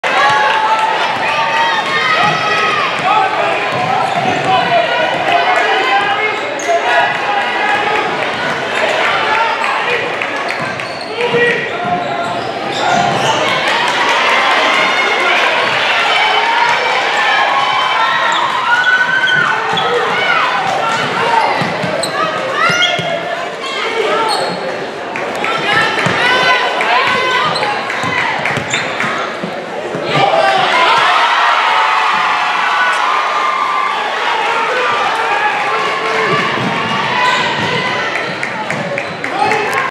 Ursuline G Lewis gets the steal and F Gordy drives past her defender for the bucket
Ursuline #1 G Alisha Lewis gets the steal and #14 F Kryshell Gordy drives past her defender for the bucket